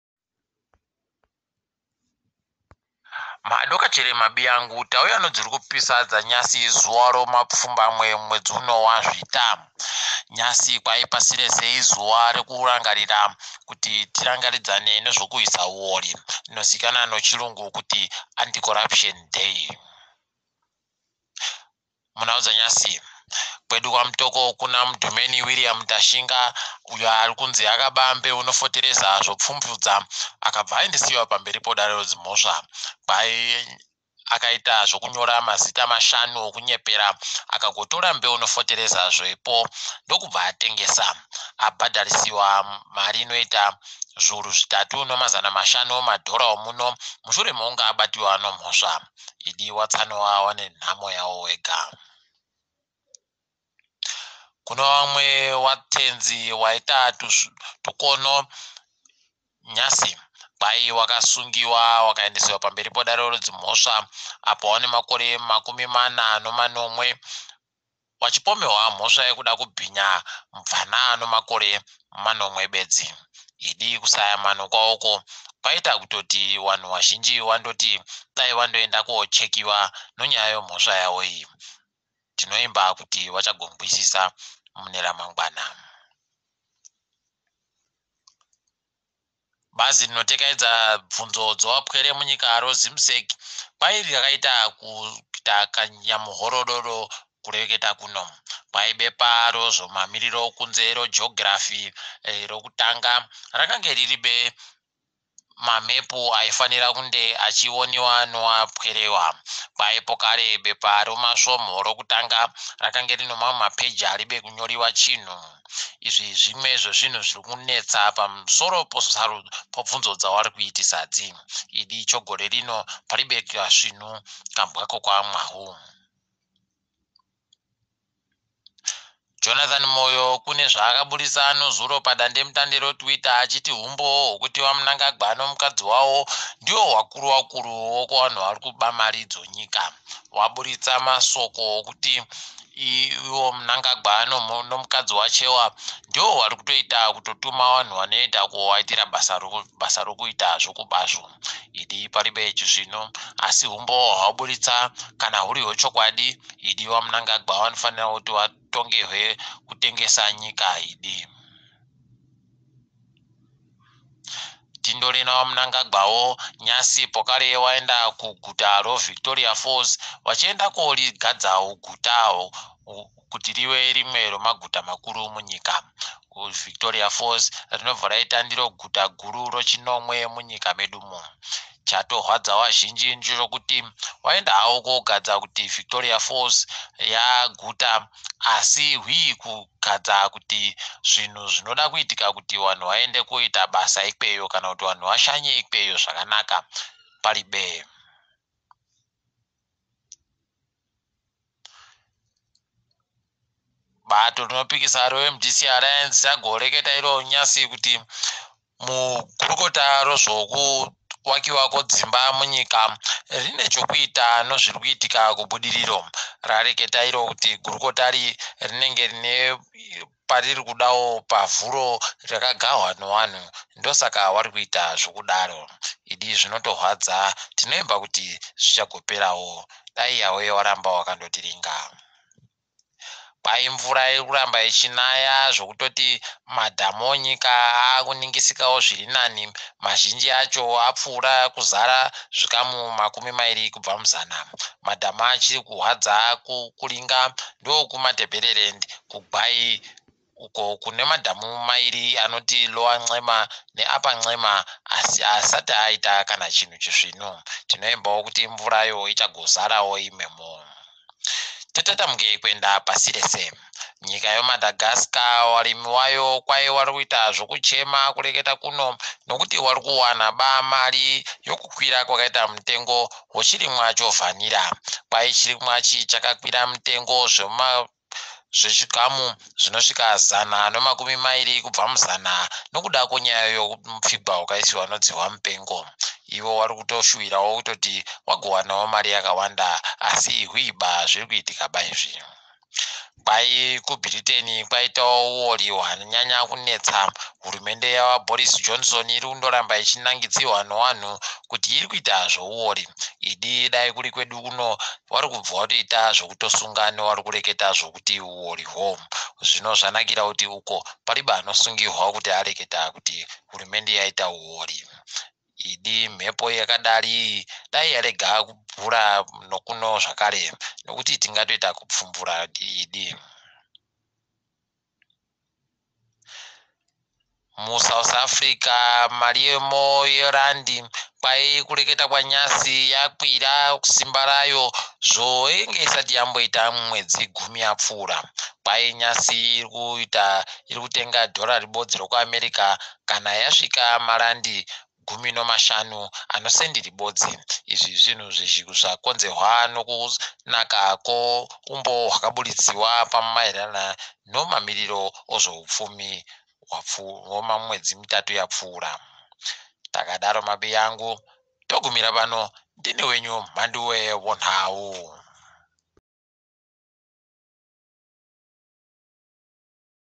Mudhumeni awona pfumvu nombewu nofotereza zvopfumvudza
Nhau dzanyasi 09 Zvita 2020 muChiBuja.

The day's news bulletin with Mhanduwe WeNhau.